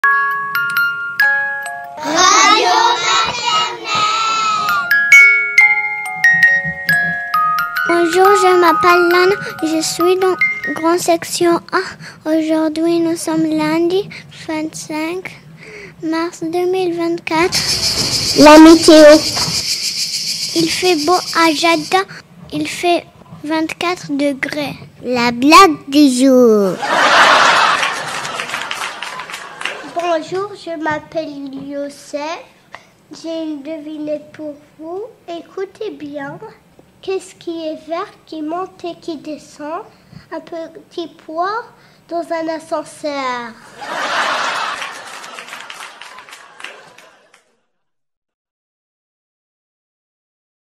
Radio maternelle du 26 mars 2024 !
Avec Youssef et Lana de la GSA !
Ecole française internationale de Djeddah